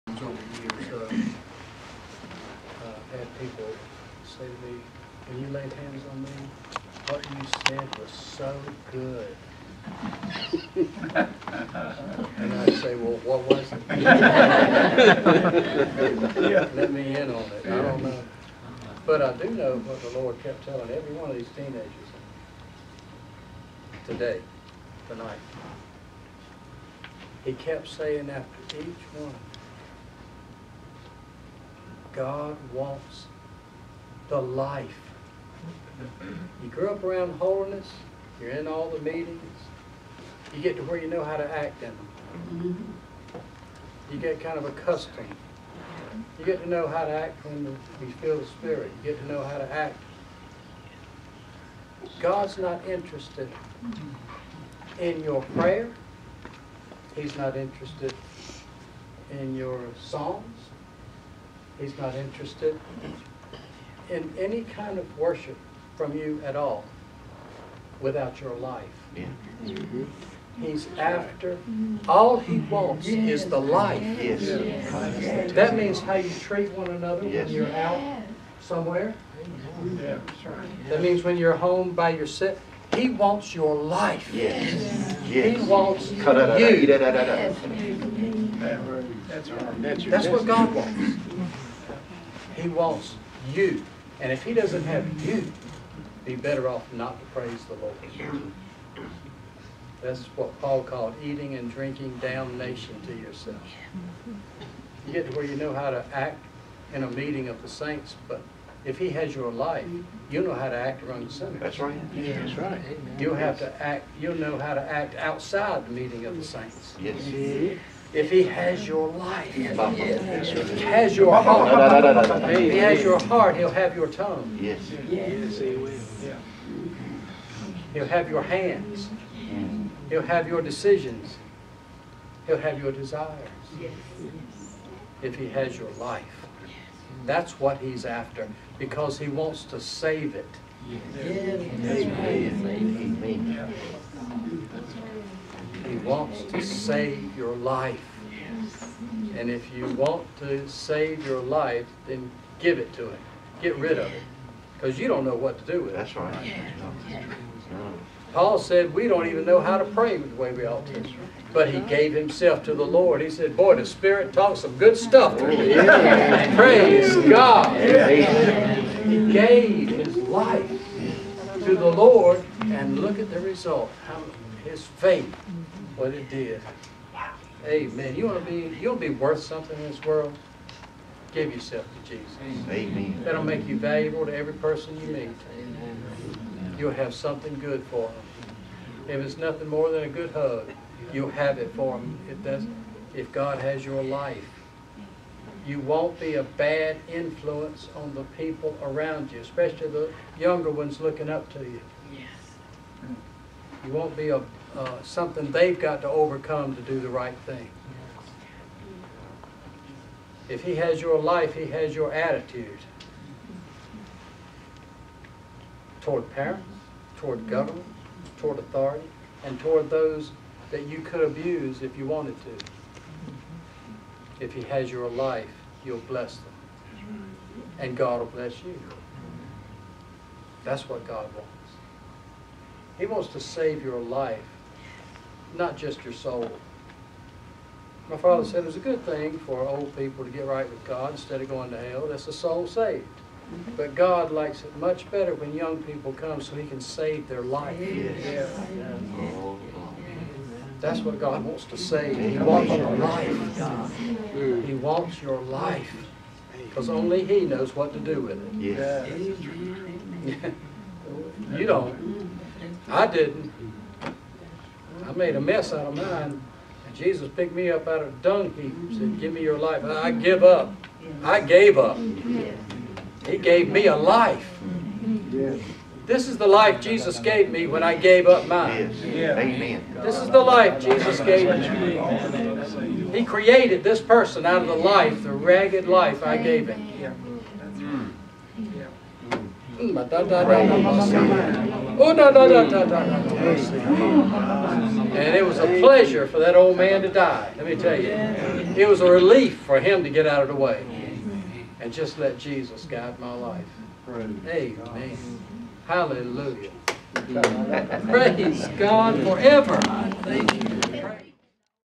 God Wants Your Life - John D. Clark, Sr.
God wants your life, so He can save it.  If you give it to Him, He will do just that.

A short message from pastor John.  From a prayer meeting in July 2019.